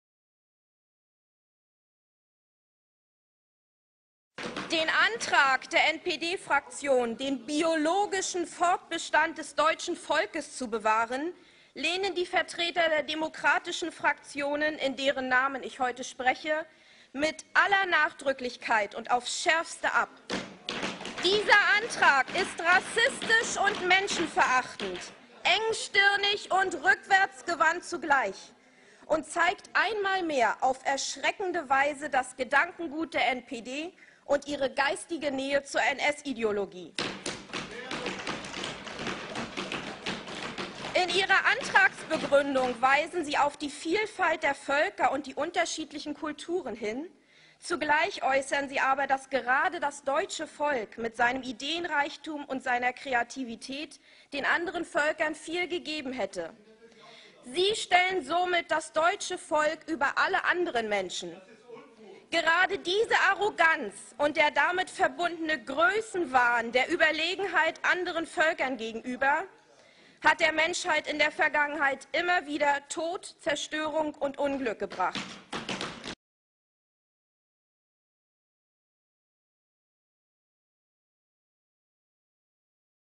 Demokraten fordern den Tod aller Deutschen !!!!!!! Antideutschismus muss bestraft werden!!!
Bitte kopieren und verbreiten!

Es lebe der Volkstod!

Die abschließende Folge der Live-Satire ist nun vollends nicht mehr zum Lachen, eher zum Zehennägeleinrollen. Die NPD stellte im Landtag von Mecklenburg-Vorpommern den Antrag, „den biologischen Fortbestand des deutschen Volkes zu bewahren", wahrscheinlich aus purer Knöpfedrückerlust. Diese Forderung ist nun für jeden historisch bewußten Menschen weder extravagant, noch zwangsläufig nationalsozialistisch. Vielmehr galt die Wohlfahrt und das Bestehen des eigenen Volkes und seiner Kultur seit jeher zu den selbstverständlichen Inhalten einer nationalstaatlichen Politik, gerade in Demokratien. Und sie ist fast überall auf der Welt die selbstverständliche Norm, nur eben dort nicht, wo der Liberalismus die Demokratie ausgehebelt hat.

Das Ergebnis kam wie erwartet, allerdings in selten schöner Reinform. Das Video wurde am 16. November 2011 aufgenommen, und Anfang Februar auf Youtube hochgeladen. Es ist auch ein Musterbeispiel jenes ritualistischen, offenbar stark lustbesetzten Empörungsdampfablassens, wie es in den Landtagen heute üblich ist. Darin erklärt Stefanie Drese, stellvertretende Fraktionsvorsitzende der SPD, geboren 1976 auf dem Staatsgebiet der DDR,  die „demokratischen Fraktionen des Landtags" (also nicht nur die SPD, sondern auch CDU, Grüne, und Linke), in deren Namen sie spräche, würden dieses Ansinnen geschlossen und „mit aller Nachdrücklichkeit und auf das Schärfste" ablehnen.

an kann getrost davon ausgehen, daß die Parteienvertreter nicht nur in Schwerin so reagieren würden. Dies ist also die Haltung der herrschenden, sich „demokratischen" nennenden Eliten der Bundesrepublik gegenüber ihrem Staatsvolk, das eben immer noch weitgehend den „Demos" stellt, was von den Gründern des Staates auch nicht anders gedacht wurde.

Warum nun diese Ablehnung „aufs Schärfste"? Weil die „demokratischen Fraktionen" so pfiffig-ironisch sind wie Christin Löchner und Begriffe wie „deutsch", „Volk" oder „biologisch" rein „dekonstruktiv" betrachten und „ad absurdum" führen wollen? Nein, weit gefehlt, sie lehnen explizit die Sache selber ab, und zwar weil (*Phrasenschweinchen an*) in den Worten Dreses ein solches Ansinnen „rassistisch und menschenverachtend, engstirnig und rückwärtsgewandt zugleich" sei.

    ... und zeigt einmal mehr auf erschreckende Weise das Gedankengut der NPD und ihre geistige Nähe zur NS-Ideologie. (...) Sie stellen somit das deutsche Volk über alle anderen Menschen. Gerade diese Arroganz und der damit verbundene Größenwahn der Überlegenheit anderen Völkern gegenüber hat der Menschheit in der Vergangenheit immer wieder Tod, Zerstörung und Unglück gebracht.

Das eigene Staatsvolk also zugrunde gehen zu lassen (der „Volkstod" oder wie immer man es nennen will, ist eine unbestreitbare demographische Tatsache, an der keine Rhetorik etwas ändern kann) wäre demnach „antirassistisch und menschenliebend, großherzig und vorwärtsgewandt zugleich", während schon das bloße Ansinnen, als Volk zu überleben und seine Eigenart sowie ein positives Selbstwertgefühl zu bewahren, Zeichen von „Arroganz", „Größenwahn" und Selbstüberhebung sei. Und diese Politik der Deutschland- und Deutschenabschaffung führe dann wohl zu Leben, Aufbau und Glück (Wer will kann hier unschwer den Schatten typischer „Umerziehungs"-Axiome erkennen, die den lange, lange nach dem Krieg geborenen Generationen in ihrer plattestmöglichen Form in Fleisch und Blut übergegangen sind.)...

Ach was, ich gebe es auf. Ich werde dergleichen nicht mehr kommentieren. Es passiert jeden Tag, und es fällt kaum mehr jemandem auf, weil eine massenwahnartige Demenz um sich greift, innerhalb derer niemand mehr klar sehen kann. Wir kochen schon längst wie der Frosch im Wasser aus der bekannten „urban legend" vom Kochtopfexperiment. Wir verfaulen langsam in einer wohltemperierten Hölle, die wir immer noch für einen Badeort an der Côte d'Azur der Posthistoire halten. Gehirne wie die zitierten sind in der Überzahl und sie sind unreformierbar, unheilbar. Wen die Götter verderben wollen, den schlagen sie mit Blindheit. „La colère des imbéciles remplit le monde", schrieb Bernanos:  Die Raserei der Dummen überflutet die Welt. Sollen sie also meinetwegen endlich zugrundegehen, wenn sie sich nichts mehr wünschen als das. In Wirklichkeit sind ihre Äußerungen nicht nur ideologische Dummheiten, Infantilismen und Rasereien; sie sind auch frevelhafte Frivolitäten, die man sich leistet,"weil es die Produktionsmittel" erlauben, und denen ein göttliches Strafgericht sicher ist.